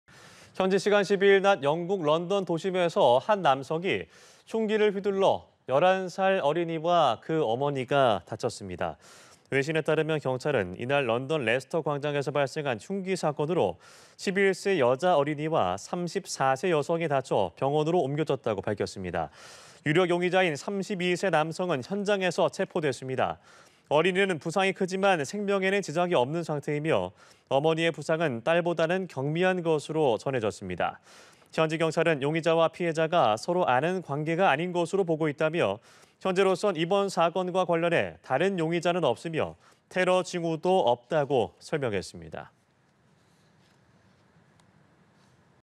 런던 도심 대낮 흉기 난동…11살 어린이·어머니 부상 / 연합뉴스TV (YonhapnewsTV)
런던 도심 대낮 흉기 난동…11살 어린이·어머니 부상

현지시간 12일 낮 영국 런던 도심에서 한 남성이 흉기를 휘둘러 11세 어린이와 그 어머니가 다쳤습니다.

외신에 따르면 경찰은 이날 런던 레스터 광장에서 발생한 흉기 사건으로 11세 여자 어린이와 34세 여성이 다쳐 병원으로 옮겨졌다고 밝혔습니다.

유력 용의자인 32세 남성은 현장에서 체포됐습니다.

어린이는 부상이 크지만 생명에는 지장이 없는 상태며, 어머니의 부상은 딸보다는 경미한 것으로 전해졌습니다.

현지 경찰은 용의자와 피해자가 서로 아는 관계가 아닌 것으로 보고 있다며 현재로선 이번 사건과 관련해 다른 용의자는 없으며 테러 징후도 없다고 설명했습니다.